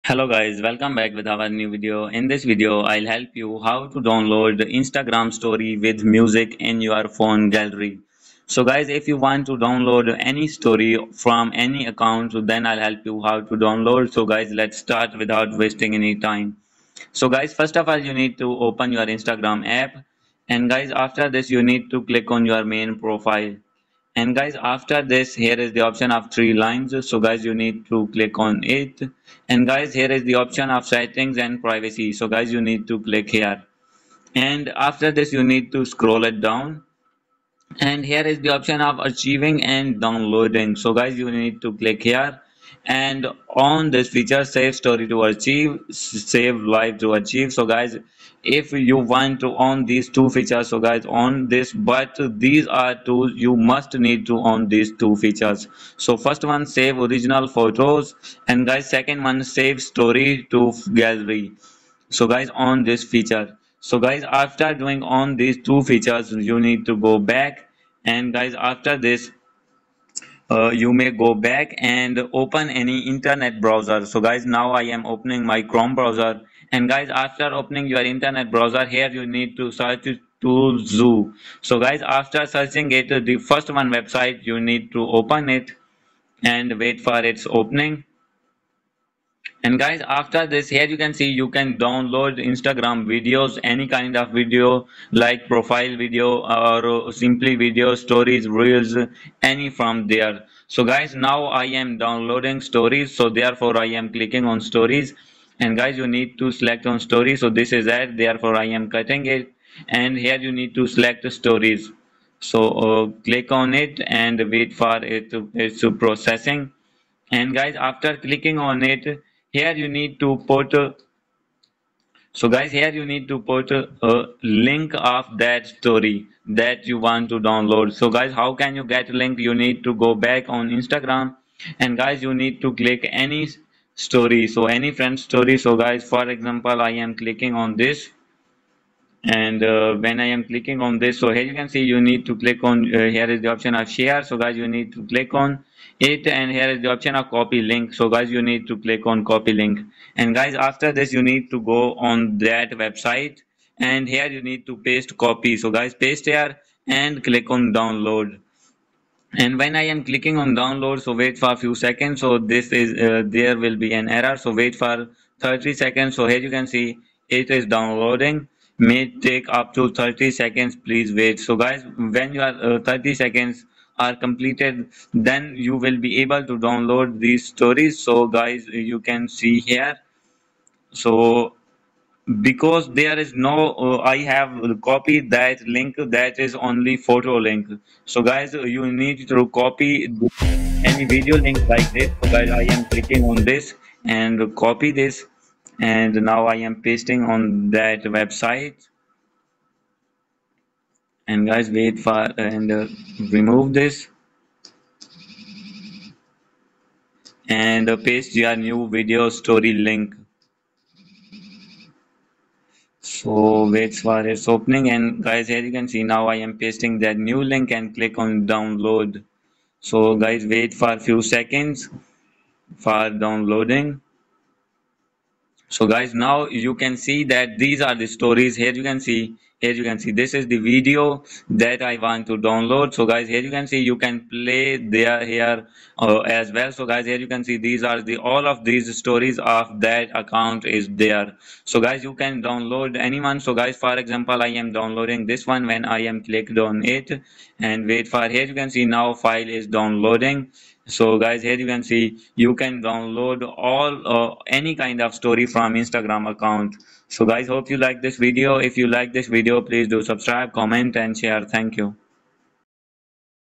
How To Download Instagram Story With Music | Download Stories On Instagram New Update
In this video, I will help you to download stories on Instagram. Please watch till the end.